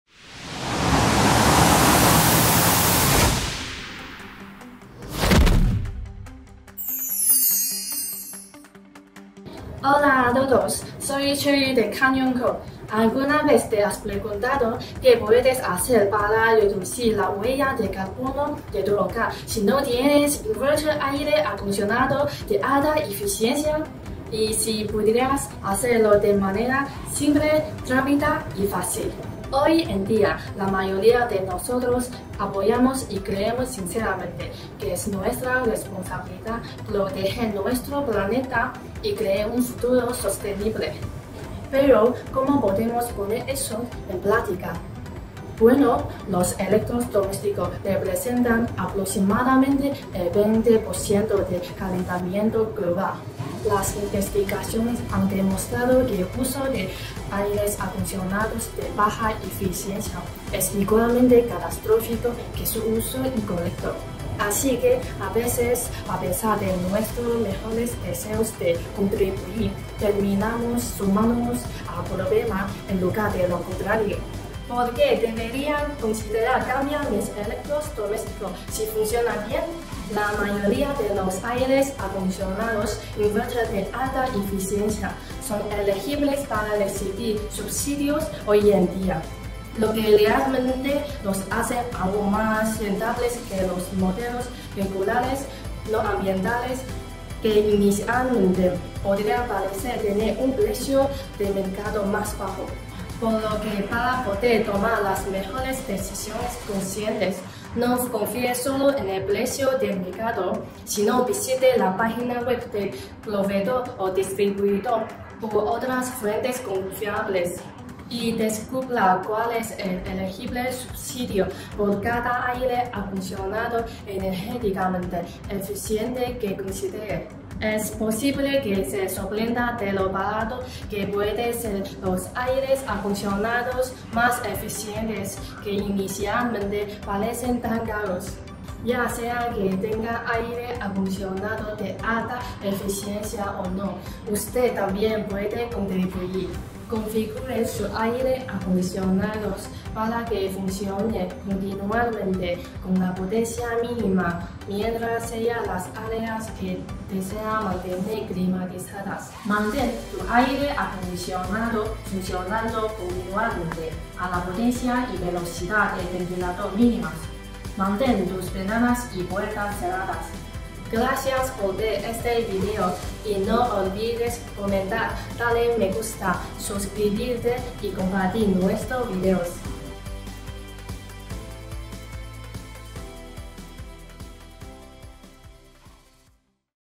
¿Por qué debería considerar cambiar mis electrodomésticos si funcionan bien?
¡Hola a todos! Soy Cherry de Kanion Co. Hoy quiero hablarles sobre cómo reducir la huella de carbono de su hogar, incluso si no tienen un aire acondicionado inverter de alta eficiencia. Aquí les dejo algunos puntos clave:

Los electrodomésticos representan aproximadamente el 20% del calentamiento global, por lo que es importante tomar medidas para reducir su impacto.

El uso de aires acondicionados de baja eficiencia contribuye al calentamiento global de la misma manera que su uso incorrecto. Es importante tener esto en cuenta.

Muchos aires acondicionados inverter de alta eficiencia son elegibles para recibir subsidios en la actualidad. Esto los hace aún más rentables que los modelos regulares, que pueden parecer más baratos inicialmente.

No se dejen engañar por el precio de mercado. Visiten la página web del proveedor o distribuidor y busquen información confiable sobre los subsidios disponibles para los aires acondicionados energéticamente eficientes que estén considerando.

Es posible que se sorprendan de lo económicos que pueden ser los aires acondicionados más eficientes a largo plazo.

Además, pueden configurar su aire acondicionado para que funcione continuamente con la potencia mínima mientras sellan las áreas que desean mantener climatizadas. Mantengan las ventanas y puertas cerradas para maximizar la eficiencia.

En resumen, aunque no tengan un aire acondicionado inverter de alta eficiencia, aún pueden tomar medidas para reducir su huella de carbono. ¡Recuerden considerar los subsidios disponibles y configurar su aire acondicionado de manera eficiente! 🌿🌍